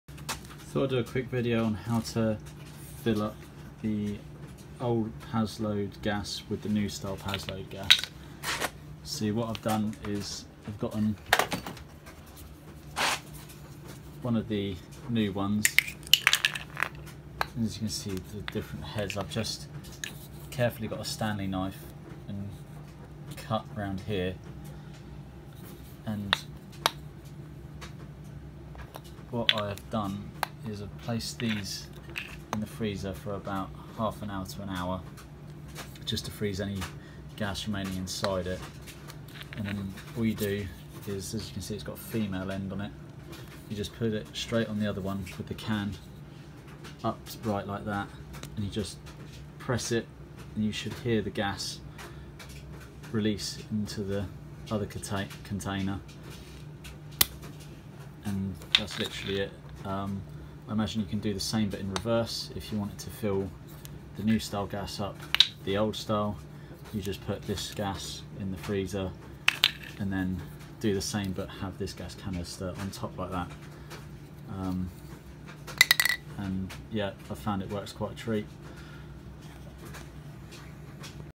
Hack How to Refill paslode im350 gas with im360ci
I found a simple way to refill old paslode gas with the new series gas as I changed guns and had a load left over. I forgot to mention in the video. You may find you have to do it in two stages to get a completely full canister, if it doesn’t completely fill up. You will need to put the gas back in the freezer again.
You will hear a slight hissing noise when filling the canister is filling up, just like if you were refilling a lighter.
You should be able to reverse the process if you wanted to fill the iseries canisters up with the older 350 style gas. I hope the helps. Use a new Stanley blade when cutting the lid and go very careful.